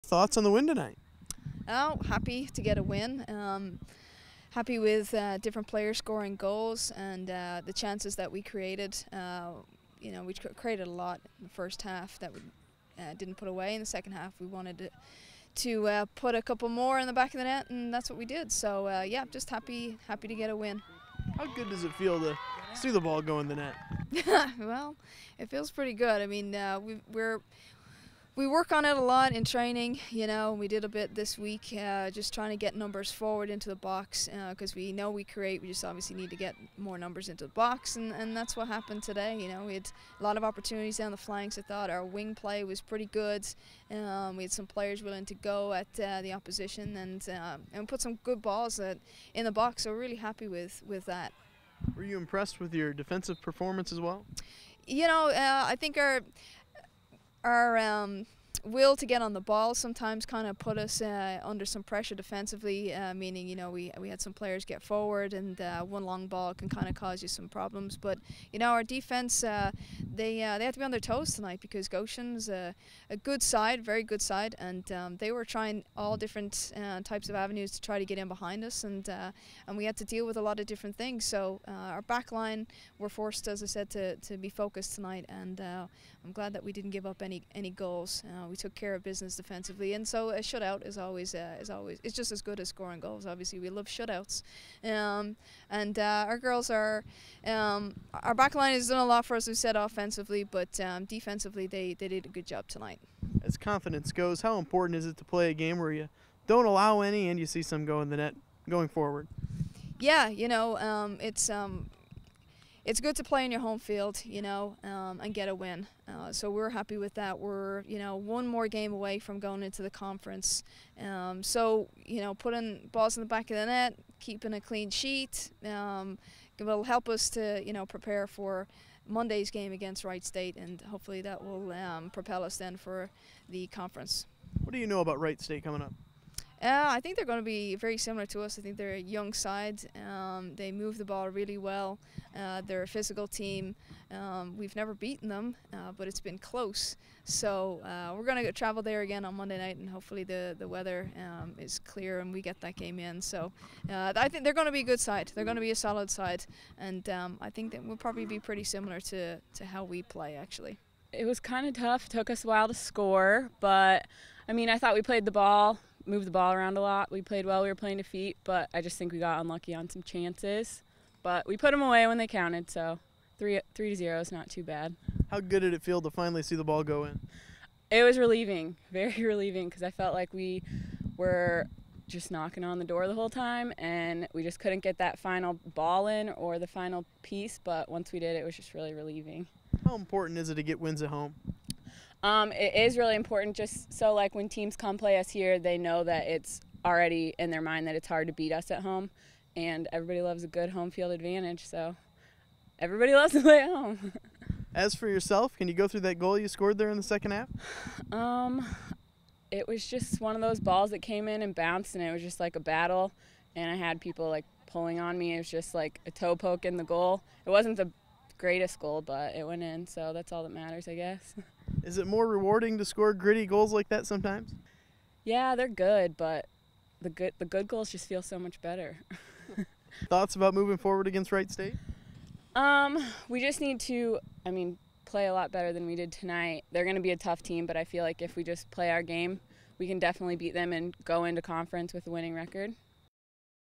9-20-12 Goshen Postgame
Head Women's Soccer Coach Margaret Saurin and Senior Forward Danielle Bishop's reaction to the Mastodons 3-0 win over Goshen.